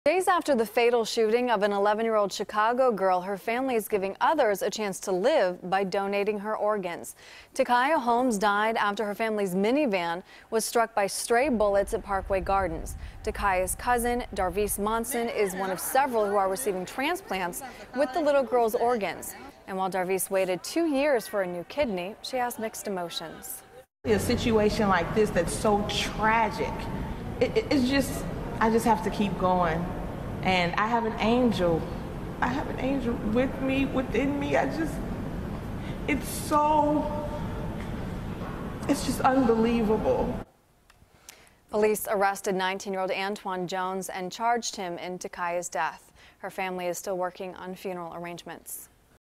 Death Of 11-Year-Old Girl Leads To 8 Saved Lives, Including Cousin
CBS 2's Erin Kennedy reports.